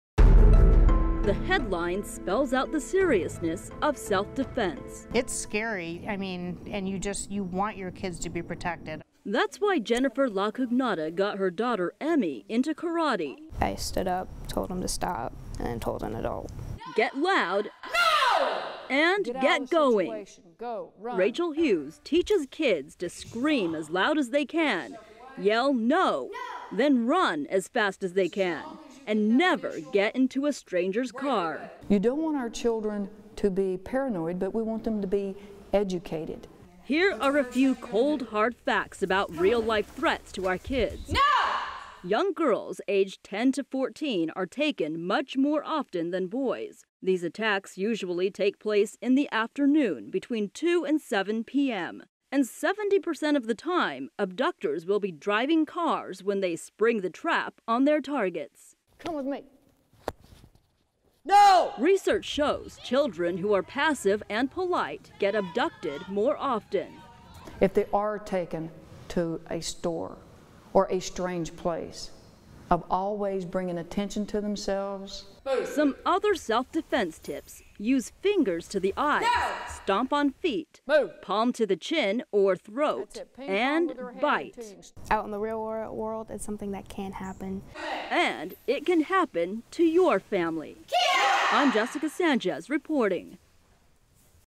KIDS ESCAPING ABDUCTORS!
At least 60,000 times a year, strangers kidnap American boys and girls. Find out what you can teach your children to lower the risk of stranger danger.